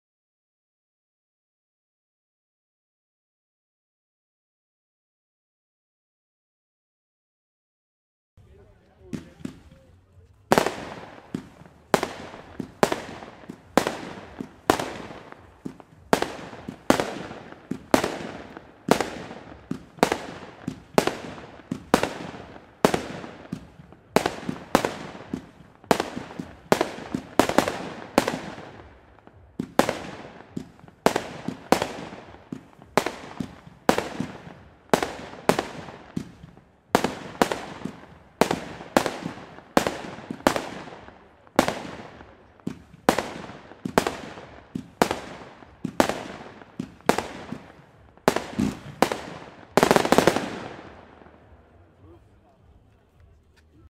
Scorpius 49Schuss Batterie von Broekhoff - Neuheit 2016
***Beschreibung Lesen***

Preis: ~25€
Braucht man nicht wirklich das Teil.